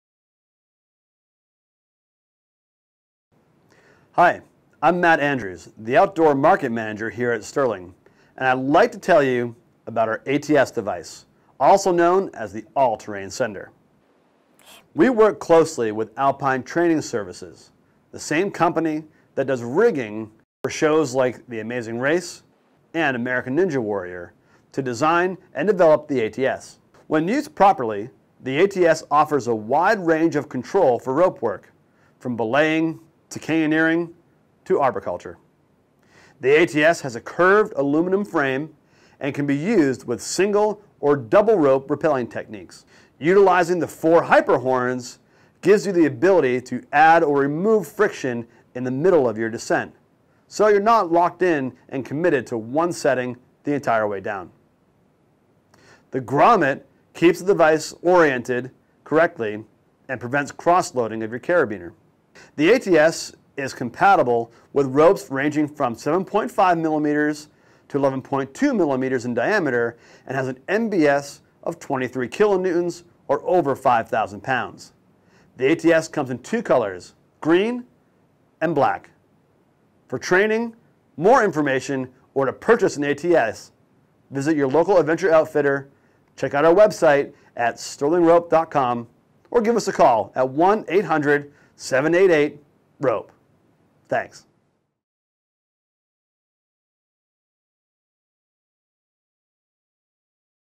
Sterling ATS Descent Device
Matt Andrews, Outdoor Market Manager introduces the ATS Device, also known as the All Terrain 'Scender. A versatile aluminum belay and rappel device that's designed for ski patrol, mountain rescue and canyoneering.